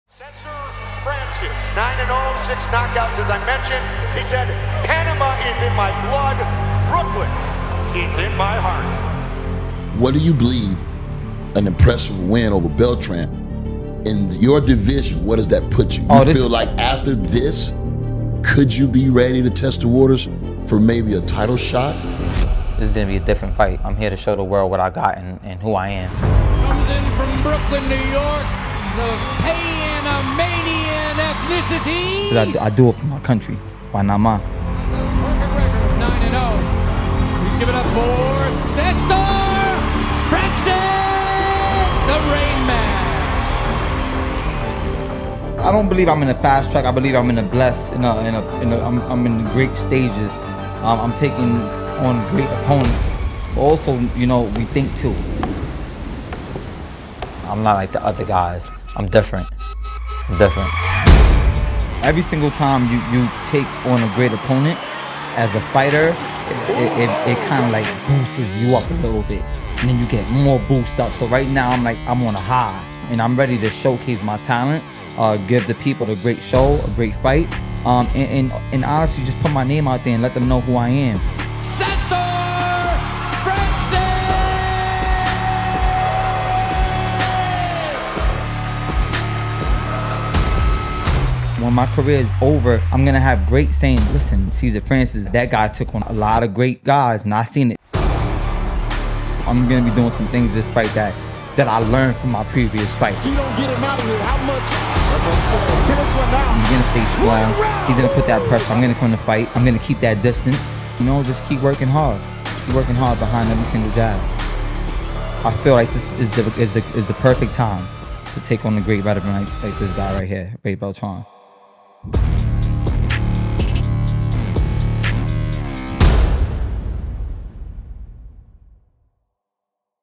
This Saturday July 9th Cesar Francis vs Raymundo Beltran in the Main event on ProboxTV.
For $1.99 a month watch live boxing on ProboxTV including this week's main event, Francis vs Beltran as the undefeated Panamainian Cesar Francis goes head to head with the Mexican veteran Ray Beltran.

Plus a great night of fights on the undercard you don't want to miss.